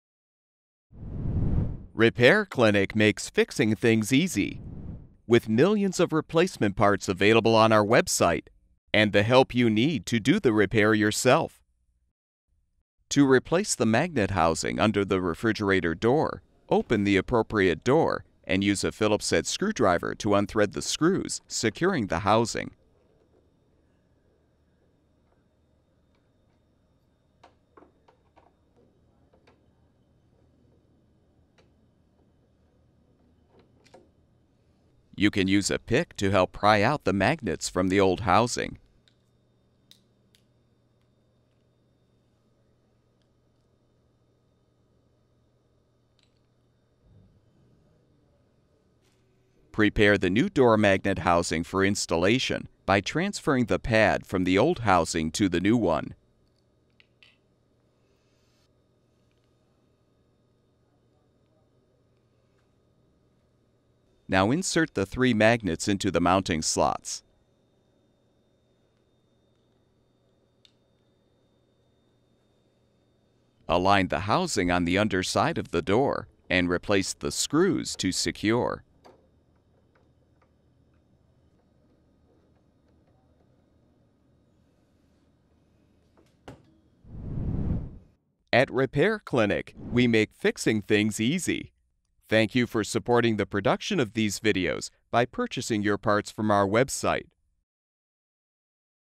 Frigidaire Refrigerator Replace Door Magnet Housing 242291001
This video provides step-by-step repair instructions for replacing the door magnet housing on a Frigidaire refrigerator. The most common reason for replacing the housing is if it is damaged, or if the door will not stay shut.

All of the information for this refrigerator magnet housing replacement video is applicable to the following brands:
Arctic Air, Crosley, Electrolux, Frigidaire, Gibson, Kelvinator, Tappan, Westinghouse

Tools needed: phillips head screwdriver